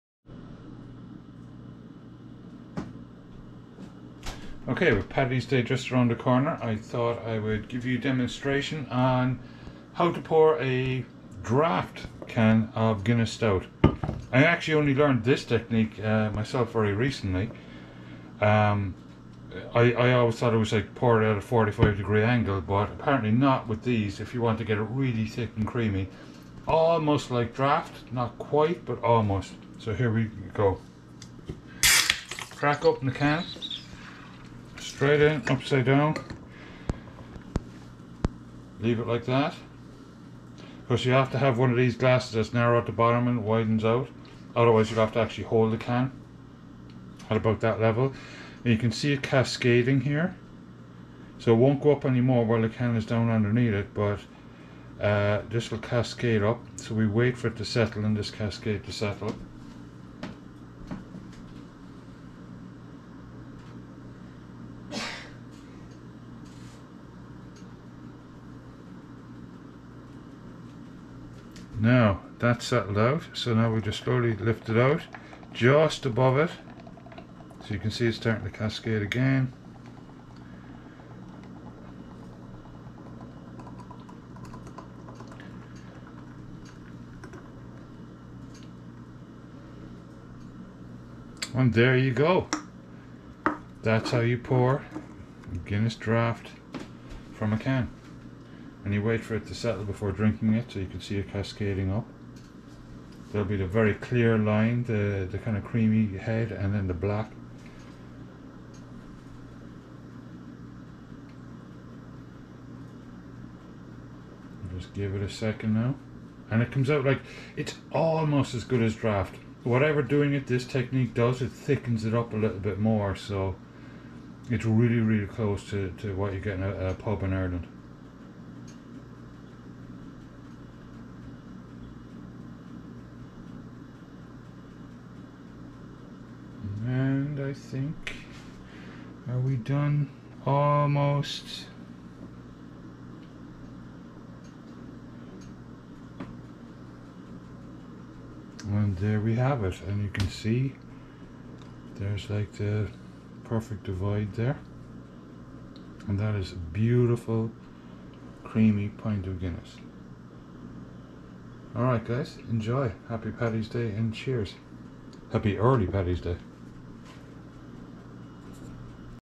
How To Pour A Can Of Guinness Draught Stout
A quick guide on how to pour a can of Guinness Draught Stout before Paddy's Day.